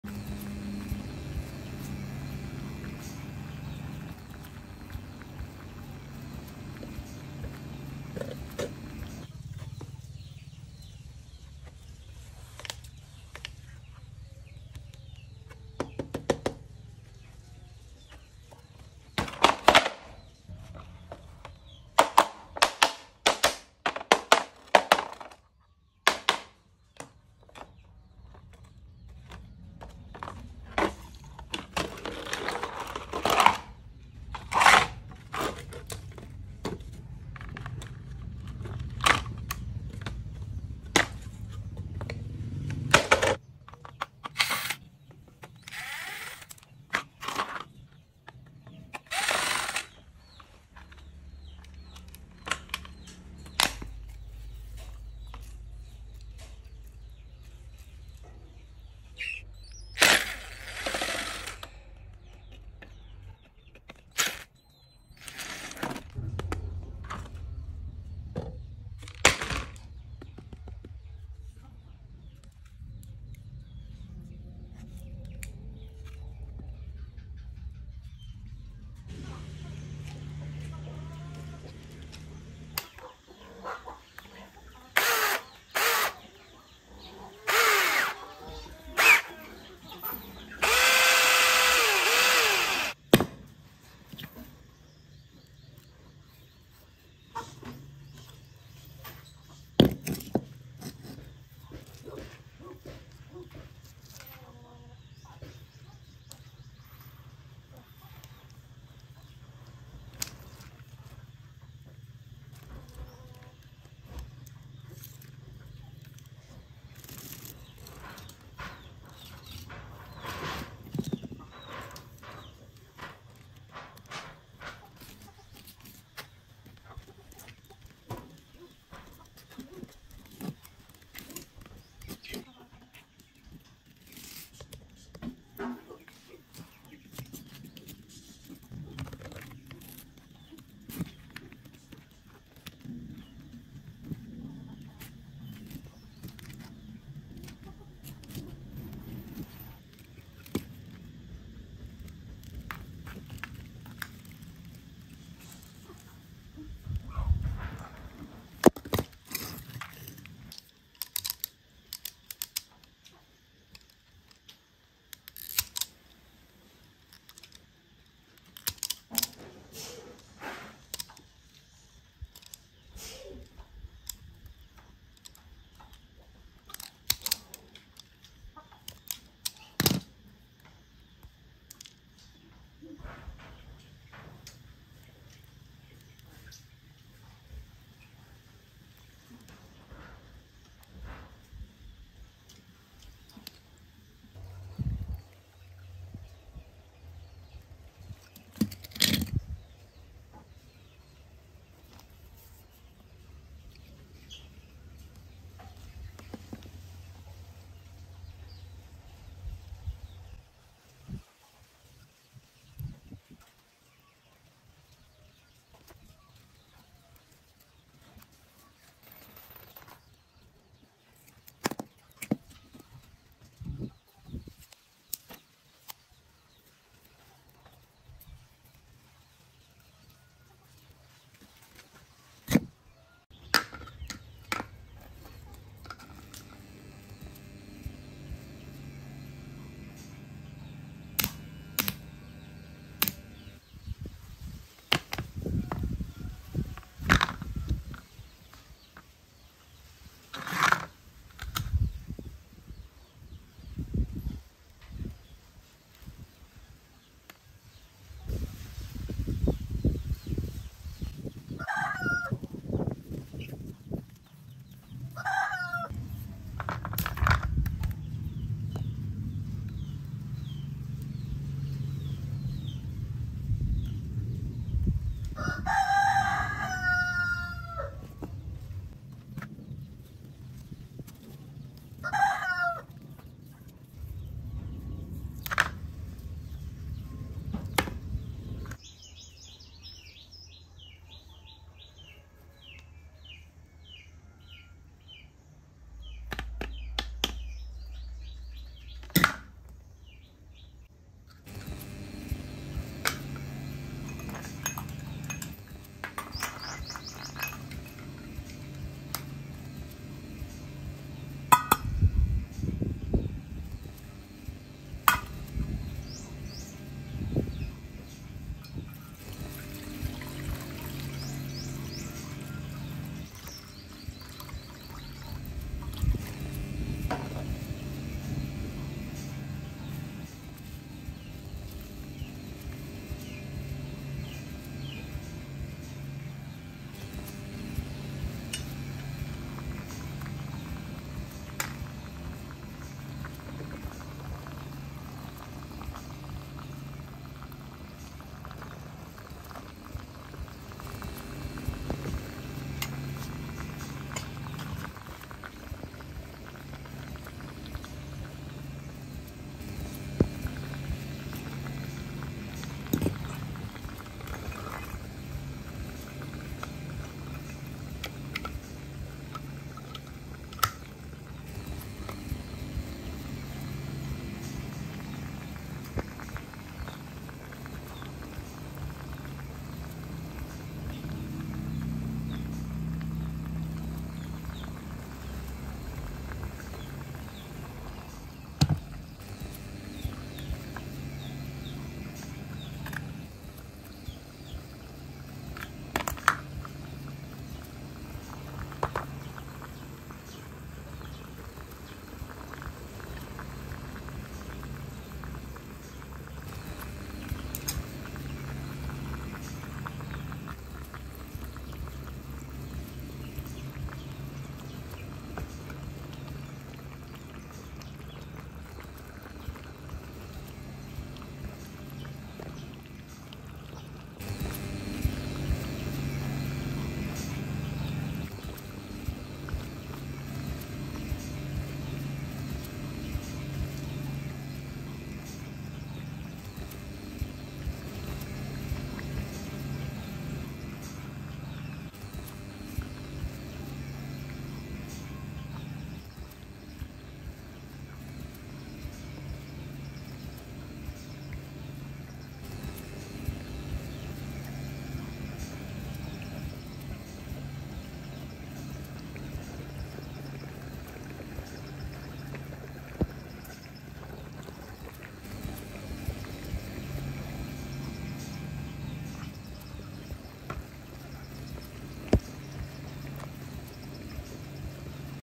I make high power electric stove from a microwave transformer | Electronic ideas
Learn how to make a high-power electric stove from a recycled microwave transformer in this DIY project! By harnessing the magnetic energy in a microwave transformer, you can create a homemade 220v generator for free electricity. This video shows you how to repurpose an old microwave transformer into a free energy device that produces enough power to cook food. Step-by-step instructions take you through modifying the transformer and adding output connections to create a high-power electric stove. This amazing project demonstrates how to tap into magnetic energy to produce limitless free electricity. With some simple rewiring, a common microwave component can be converted into a generator that makes free power for your stove, tools, lights and more. Using a transformer, some wire and basic materials, you'll learn how to make an electric cooking device from scratch in no time!